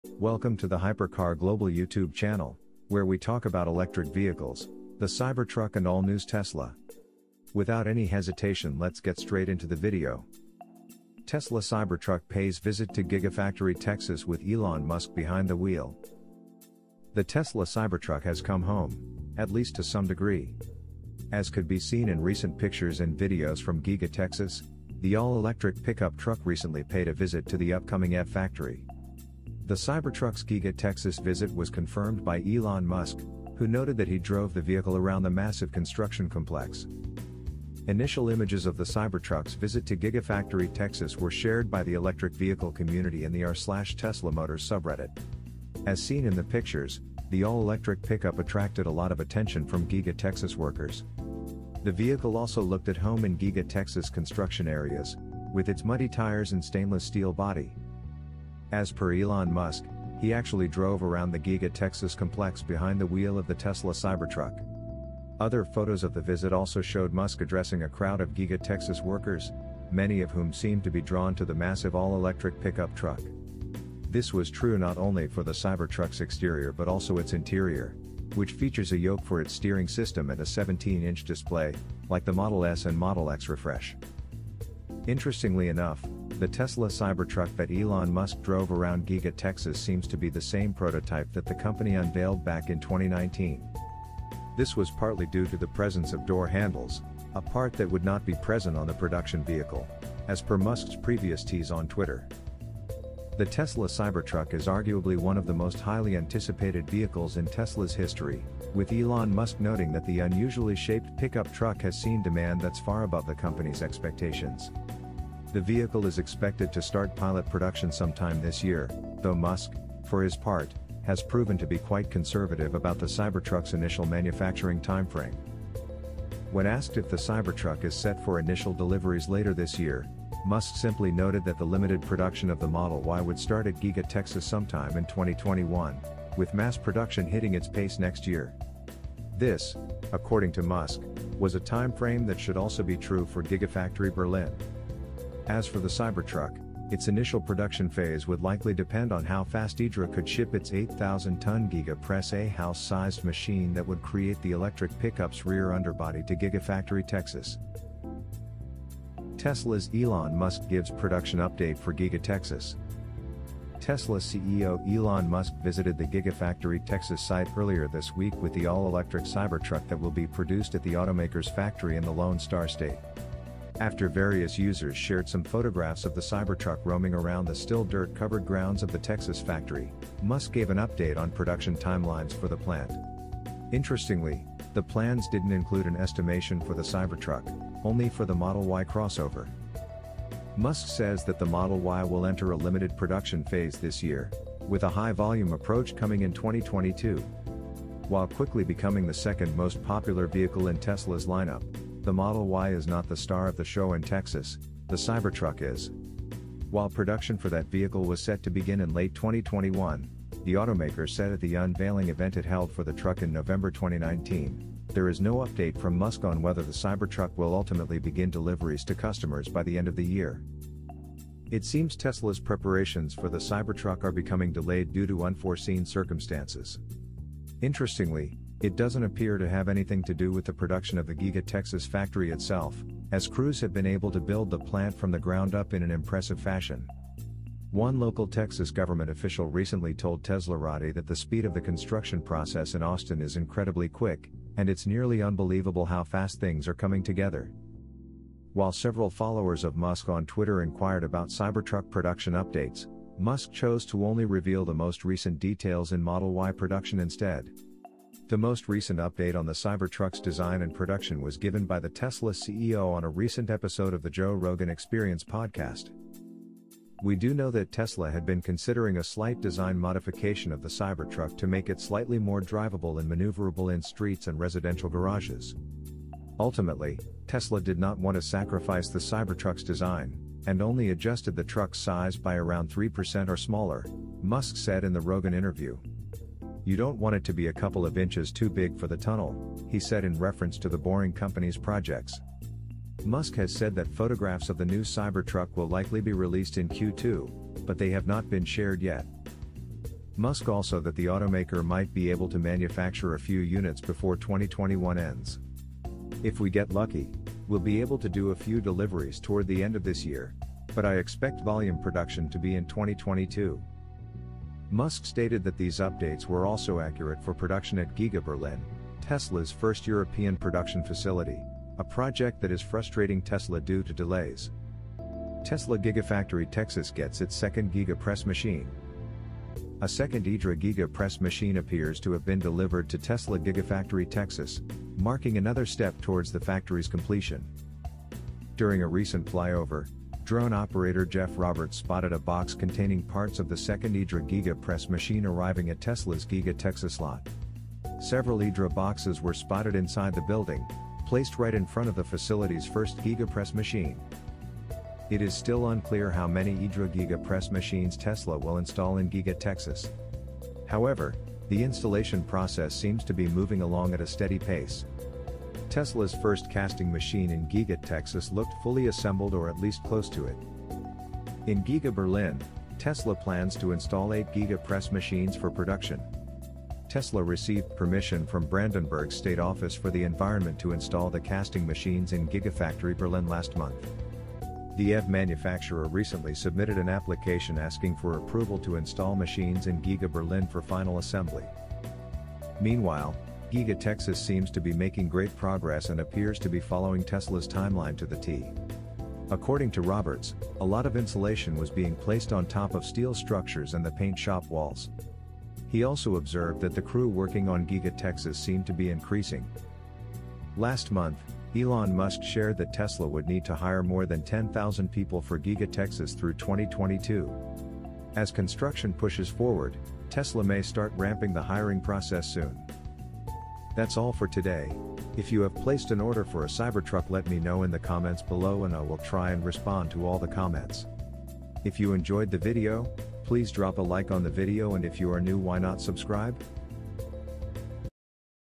Tesla Cybertruck 2021 Production News Update!
Today we take a look at the Tesla Cybertruck. This is an update on the methods used in order to build the truck. Also looking at the Giga factories and progress they've made in order to produce more cars. Second IDRA GIGA Press in Texas is also covered in this video.